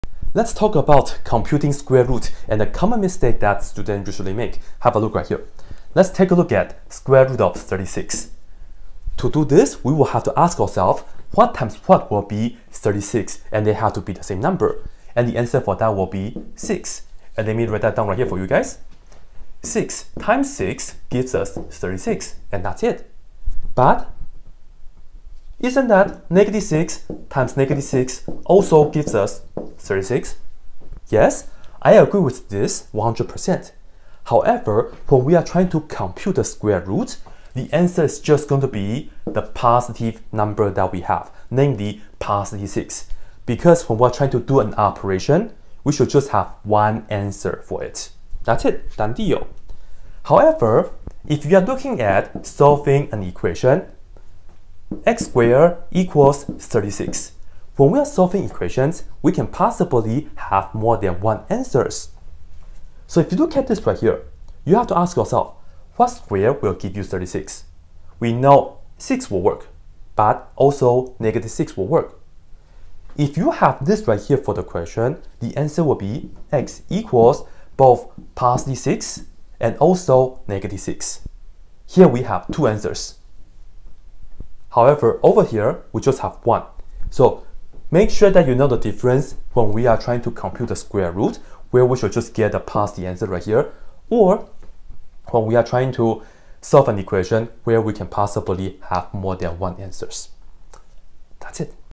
why sqrt(36) is just positive 6
When we want to evaluate the square root of a number, say 36, the answer is just positive 6. It is a common math mistake for students to say the answer is both positive 6 and negative 6. It is both positive and negative 6 when we want to solve the quadratic equation x^=36